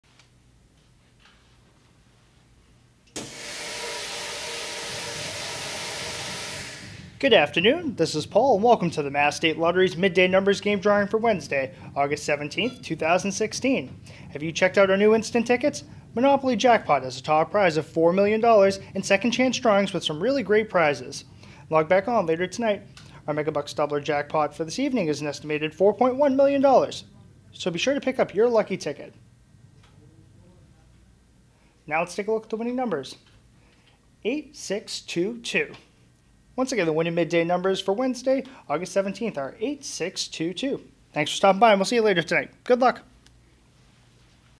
Midday Numbers Game Drawing: Wednesday, August 17, 2016
The official winning numbers in the Mass. State Lottery's Midday Numbers Game drawing for Wednesday, August 17, 2016 are: 8-6-2-2.